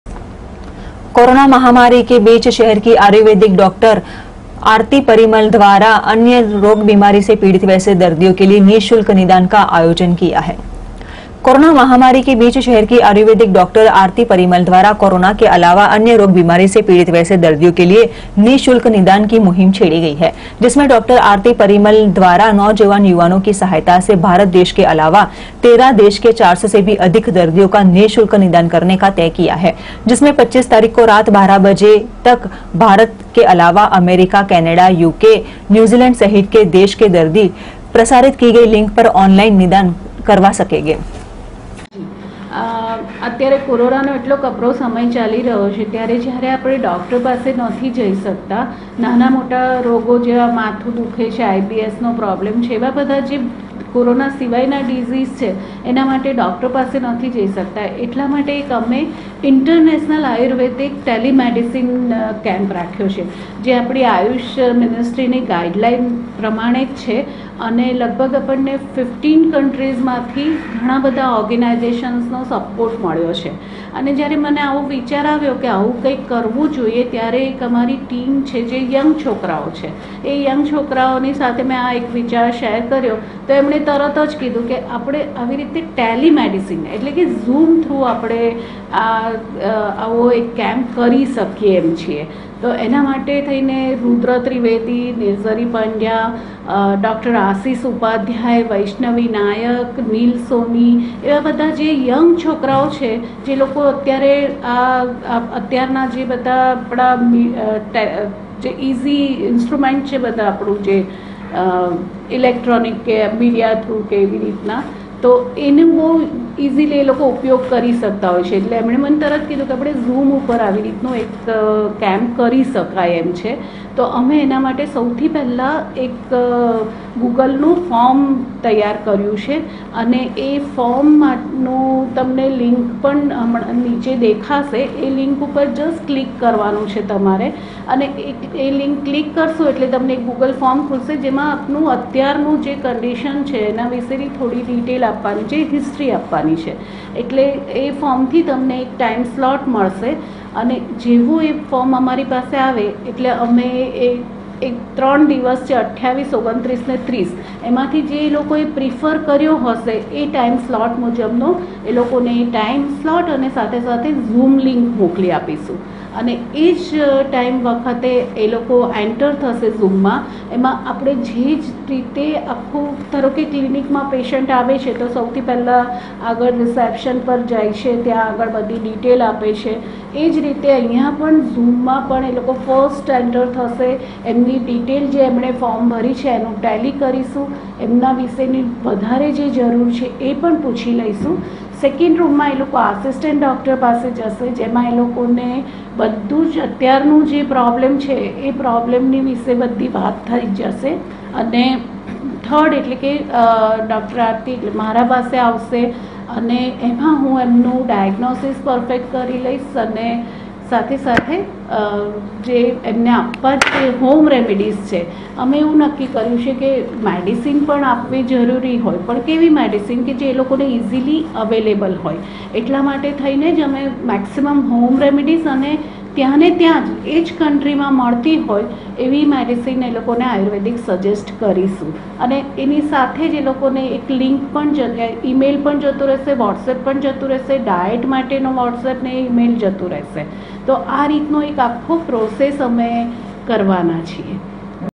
Ibn news vadodara
कोरोना महामारी अंतर्गत शहर की डॉक्टर आरती परिमल द्वारा अन्य रोग बीमारी से पीड़ित दर्दियो को निशुल्क निदान का अभियान